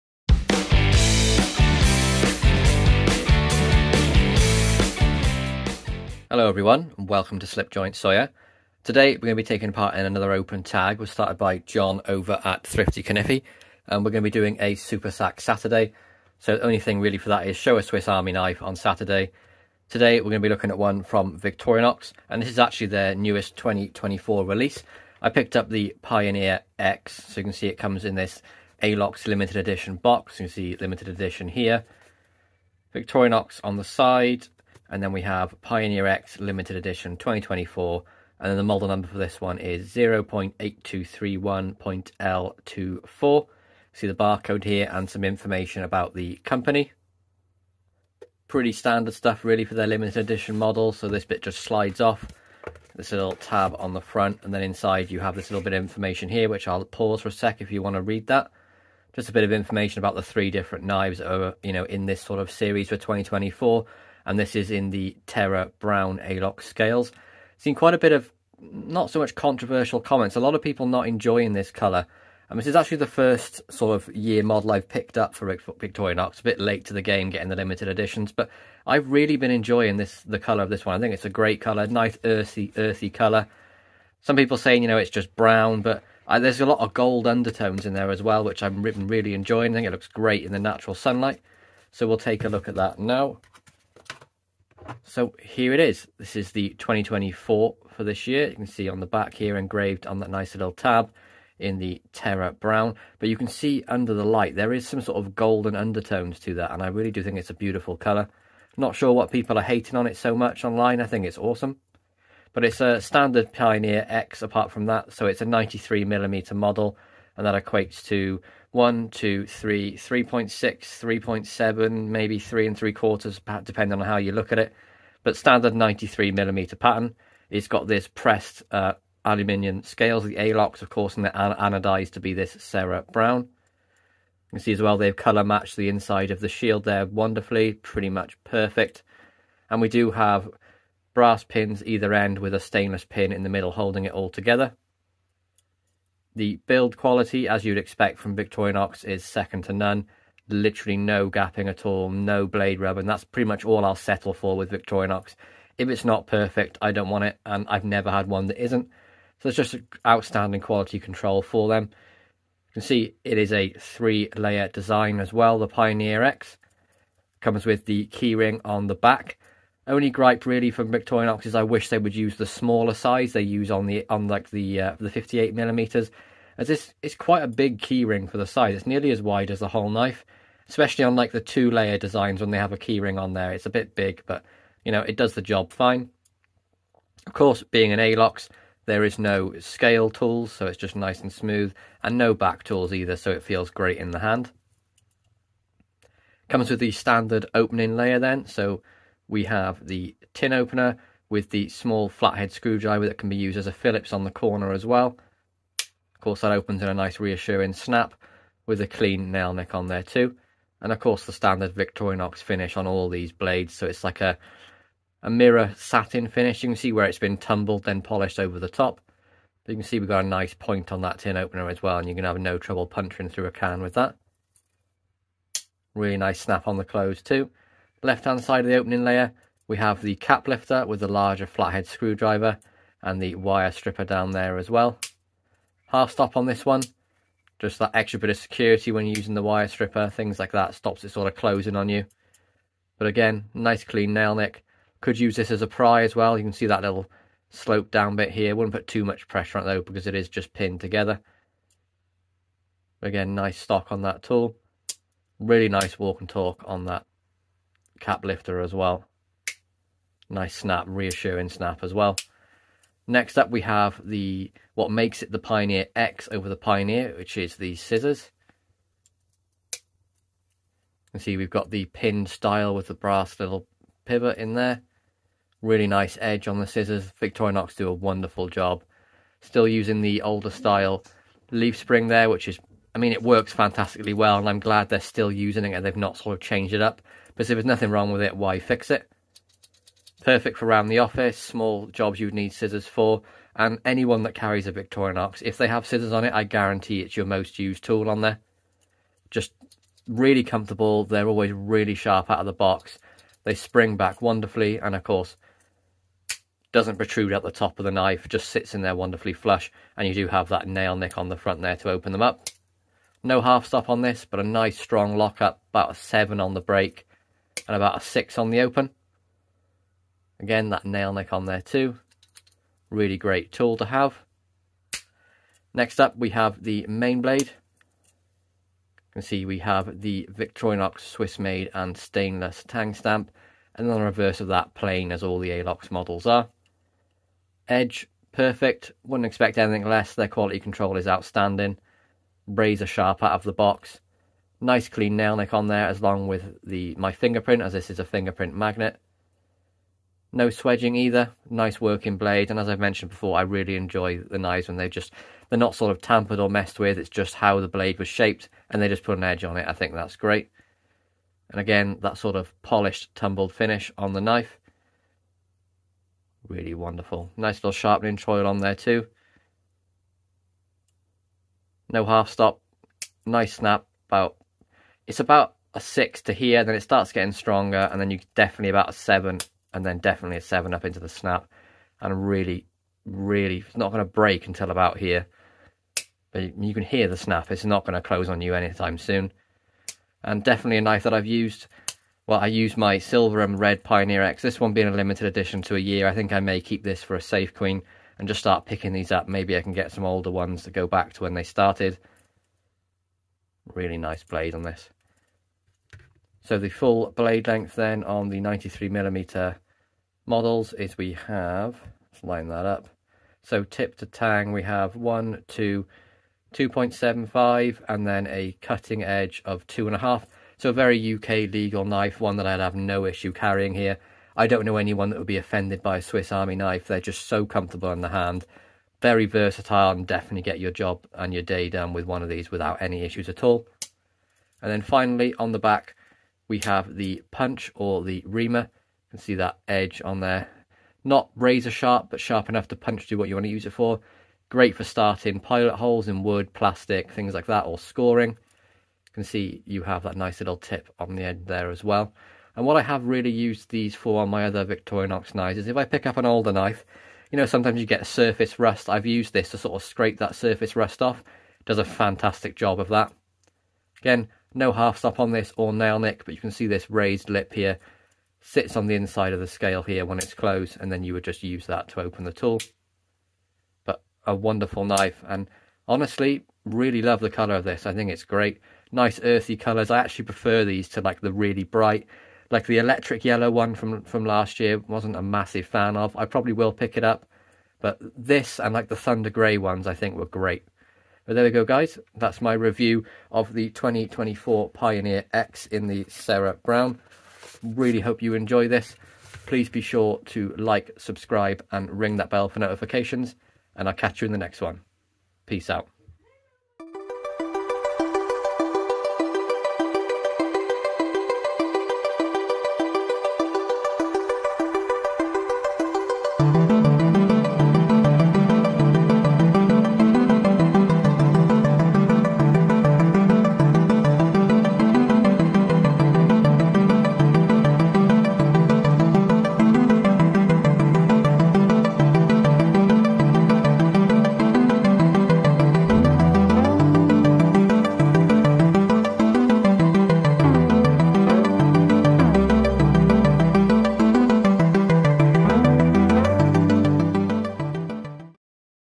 Victorinox Pioneer X 2024 Terra Brown Limited Edition Swiss Army Knife 0.8231.L24
Review of the Victorinox Pioneer X 2024 Terra Brown Limited Edition 0.8231.L24 - SuperSAKSaturday @ThriftyKaniffy

Join myself and loads of other members of the Knife Community in the Slipjoint Sanitarium Discord chat

Slipjoint Sawyer
Creator\Reviewer